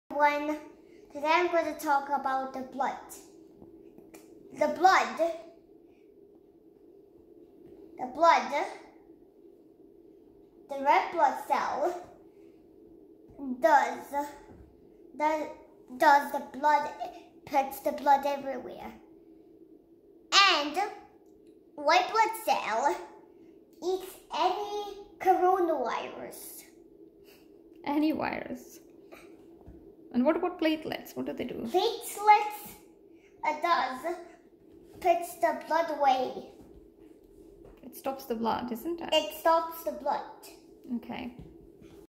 Blood cells
Jay got interested to know about why blood is red color 😂 now he knows blood cells